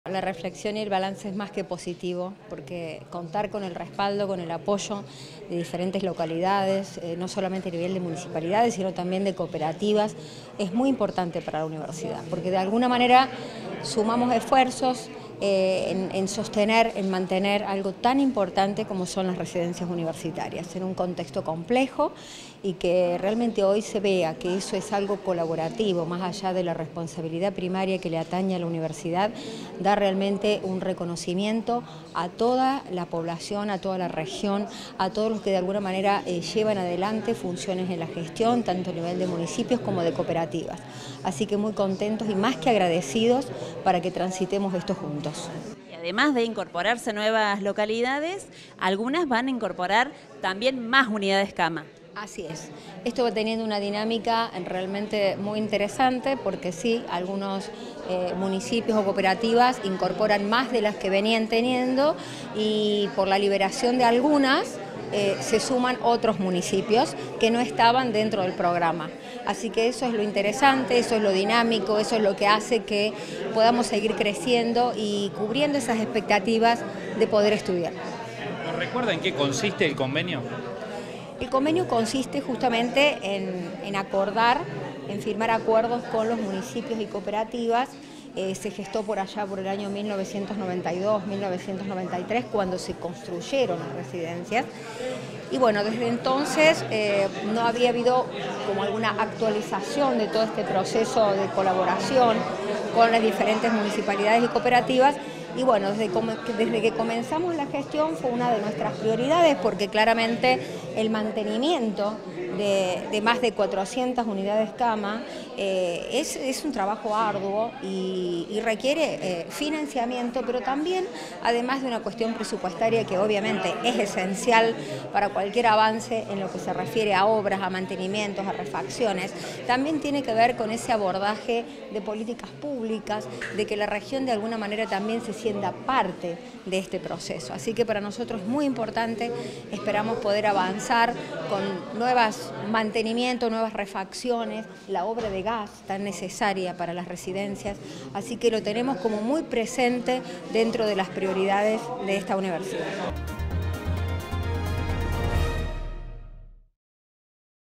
RESIDENCIAS ESTUDIANTILES: NUEVAS LOCALIDADES DE LA REGIÓN SE SUMAN AL CONVENIO CON LA UNRC
MARISA ROVERA – RECTORA
UniRio TV
Universidad Nacional de Río Cuarto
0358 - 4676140